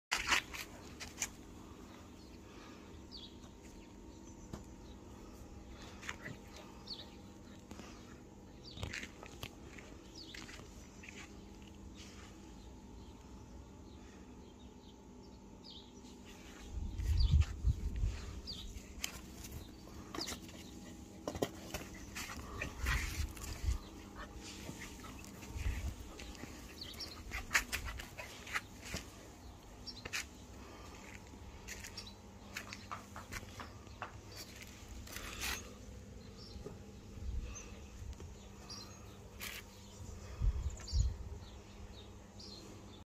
Puppy Raya loves to play in the garden!
Raya is a cute mixed puppy of a labrador and a Frisian Stabij!